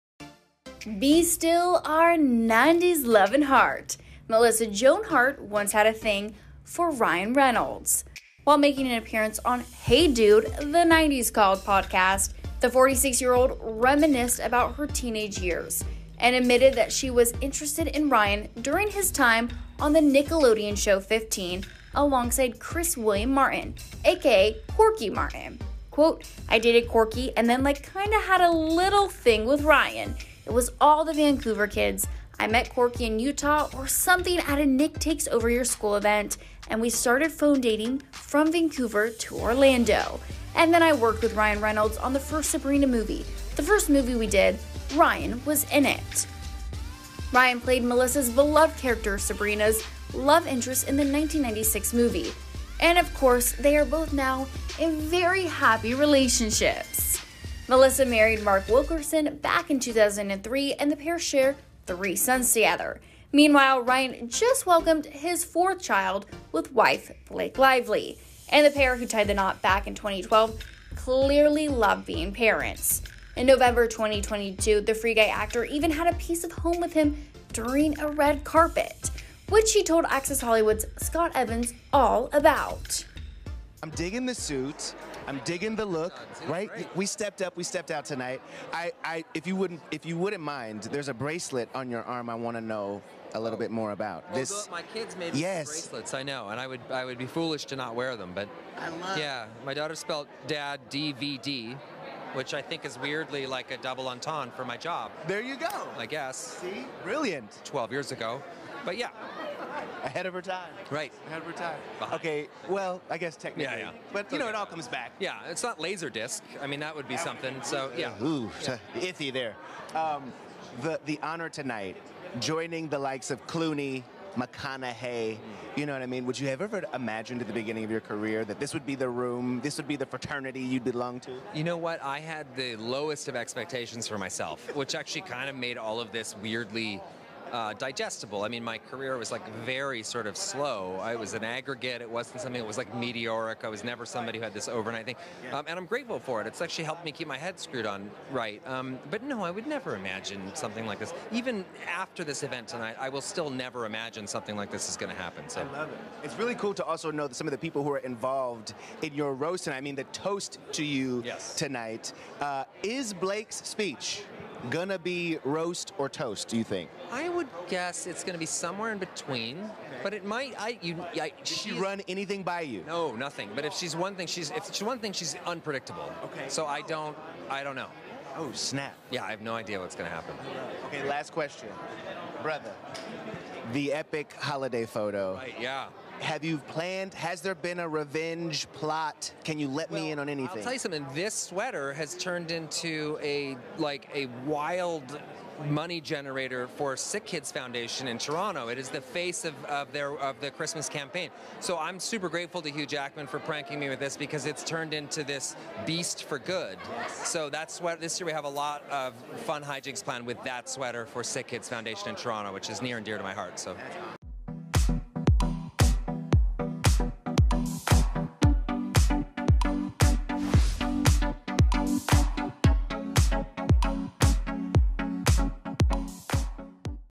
Melissa Joan Hart Had Feelings For Ryan Reynolds In The 90s'
Melissa Joan Hart once had a "thing" for Ryan Reynolds. While making an appearance on "Hey Dude...The 90s Called!" podcast, the 46-year-old reminisced about her teenage year and admitted that she was interested in Ryan back in the day.